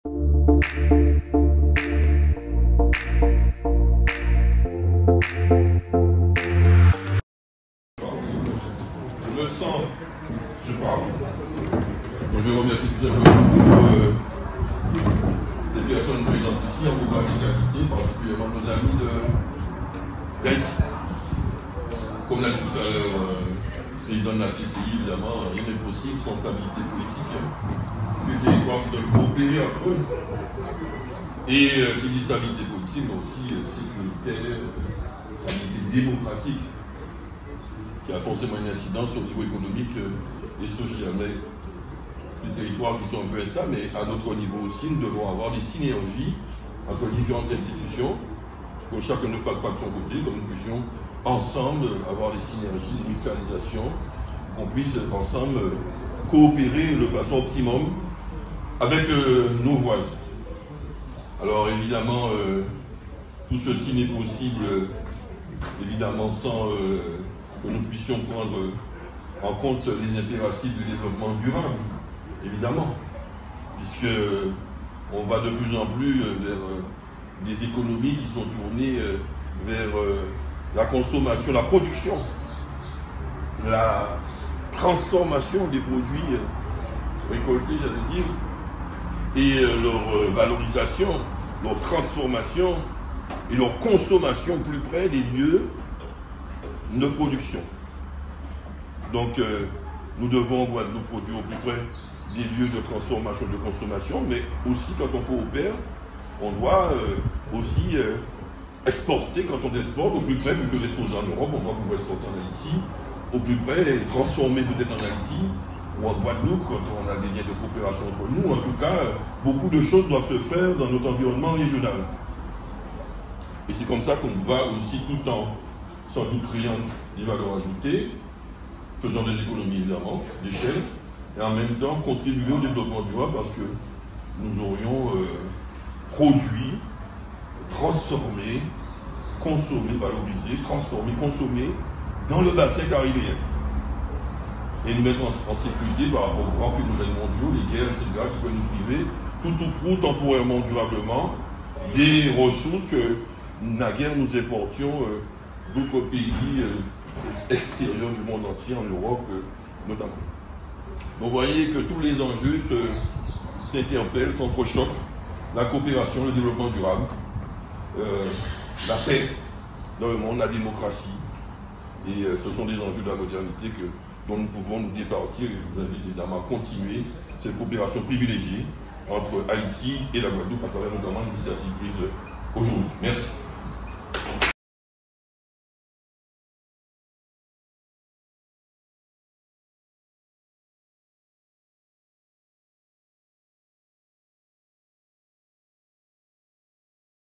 Économie Circulaire-intervention du Président E. JALTON/ Mission Coopération HAÏTI
Débat: Économie Circulaire-intervention du Président E. JALTON/ Mission Coopération HAÏTI - déchets/ 11mai2023